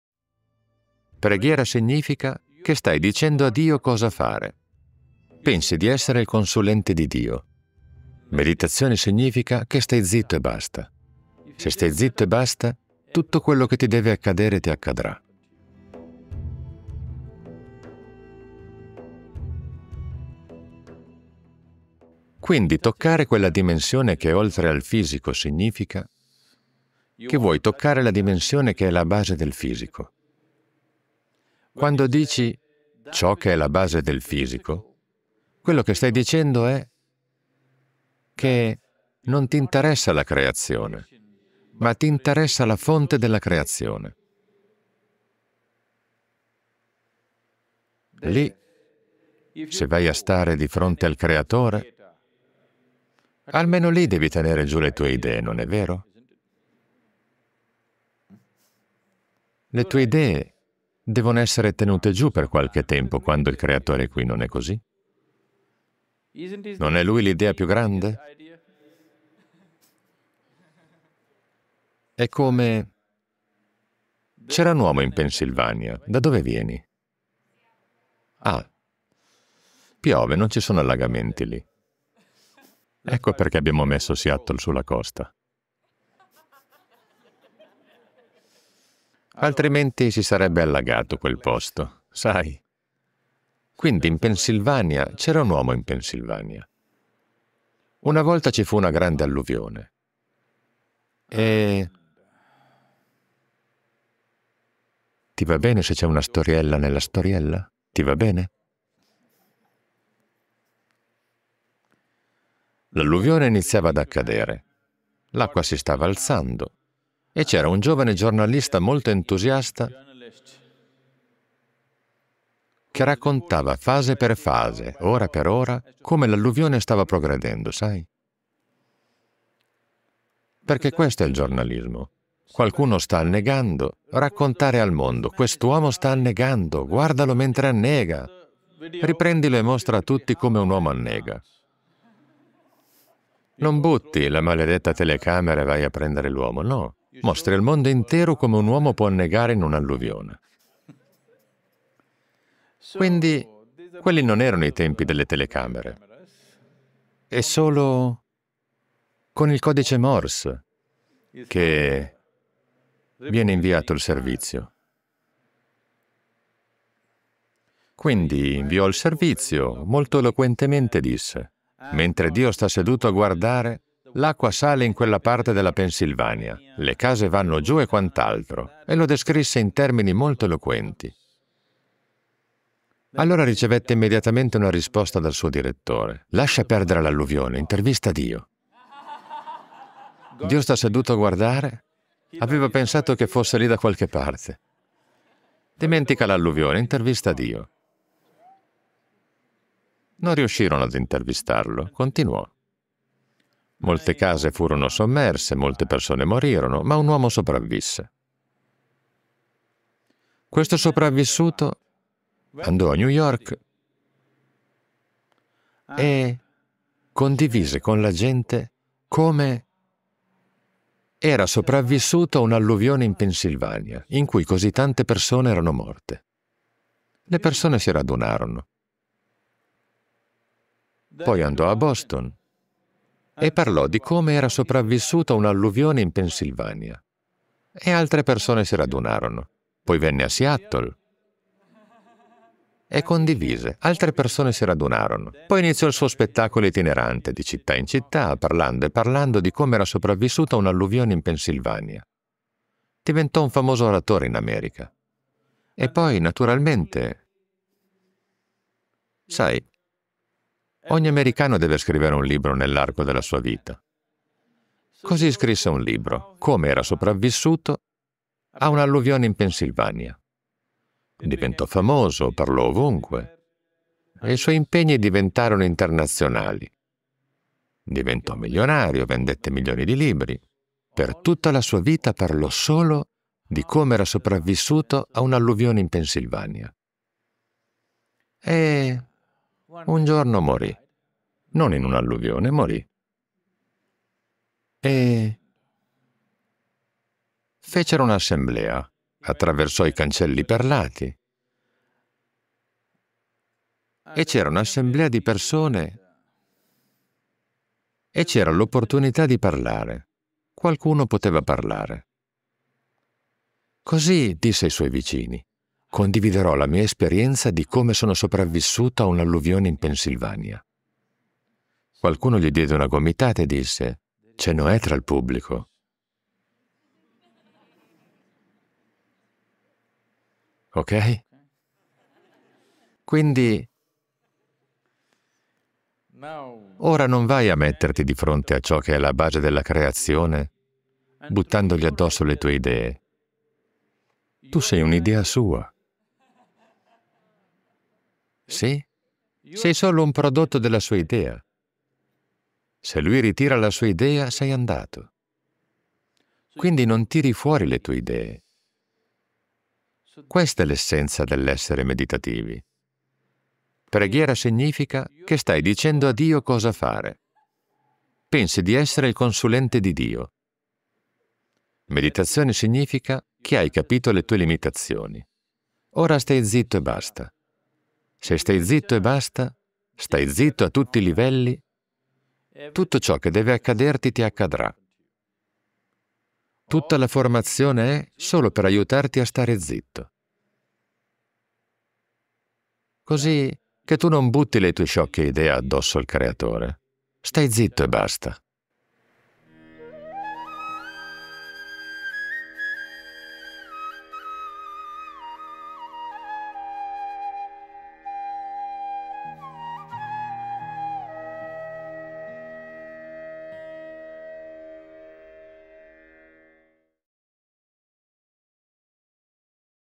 Dovresti pregare Dio? La risposta illuminante di Sadhguru | Sadhguru Italiano
Dovresti pregare Dio? La risposta di Sadhguru potrebbe essere una sorpresa totale, sia che sei tipo da preghiera che da “non preghiera”.